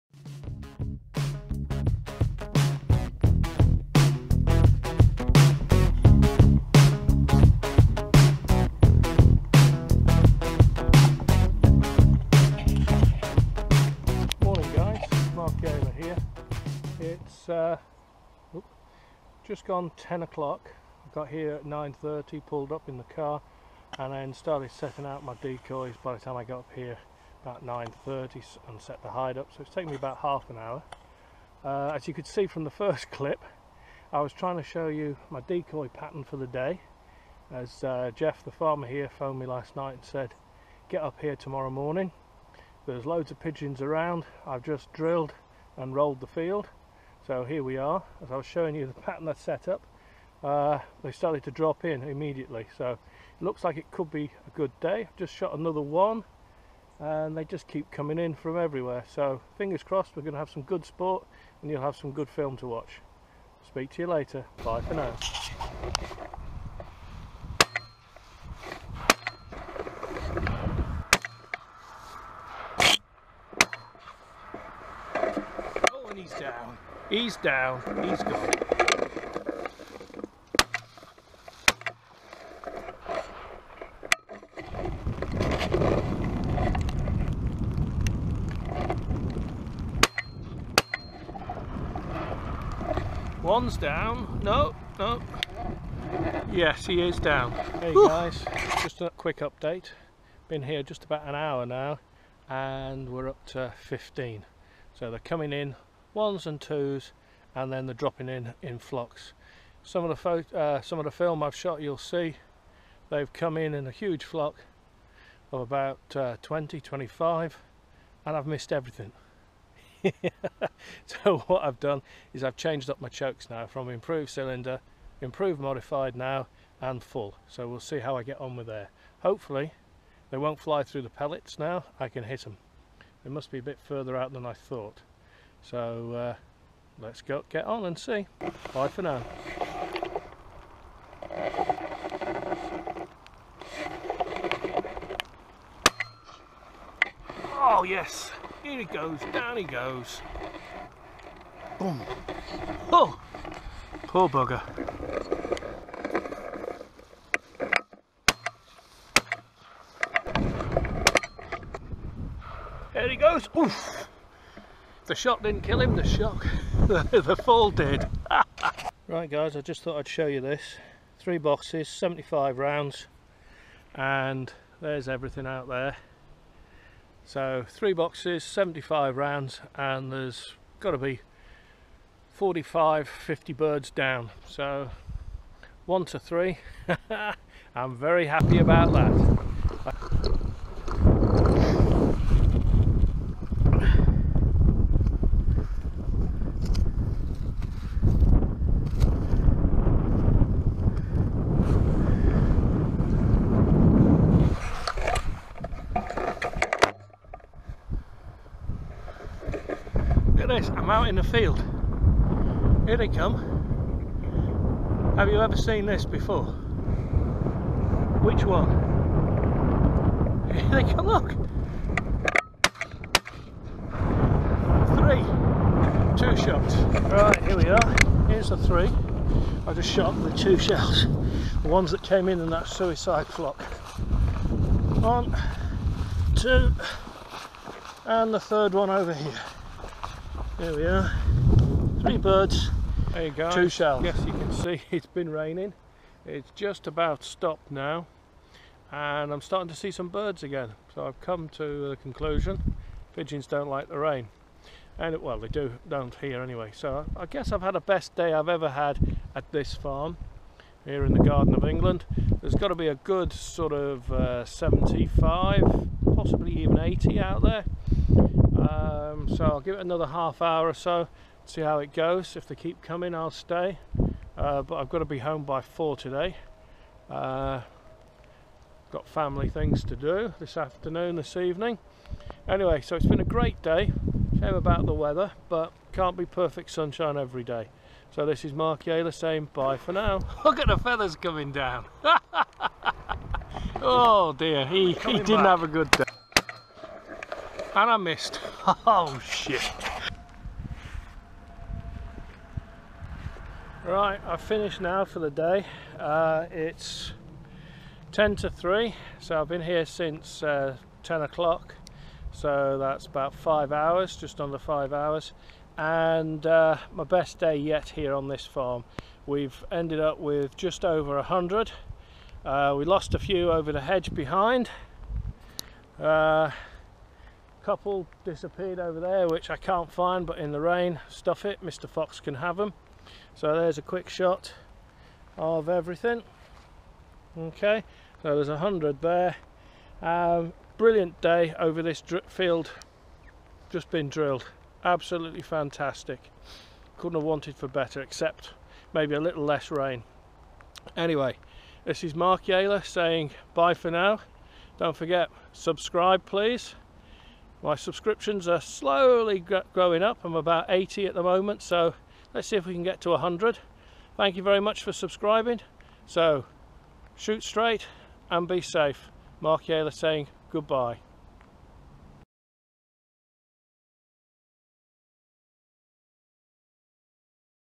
Pigeon shooting over drilled barley
My best day yet for the pigeons 120 for 245 cartridges!